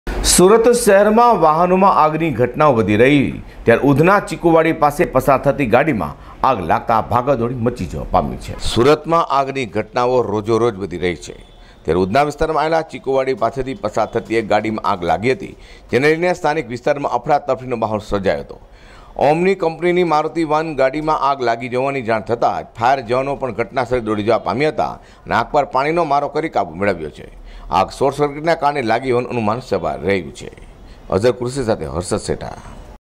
ઉધનામાં દોડતા વાહનમાં આગ લાગતા મચી ગઈ ભાગદોડ@G24News
શોર્ટ સર્કીટના કારણે વાહનમાં આગ લાગી હોવાની ચર્ચા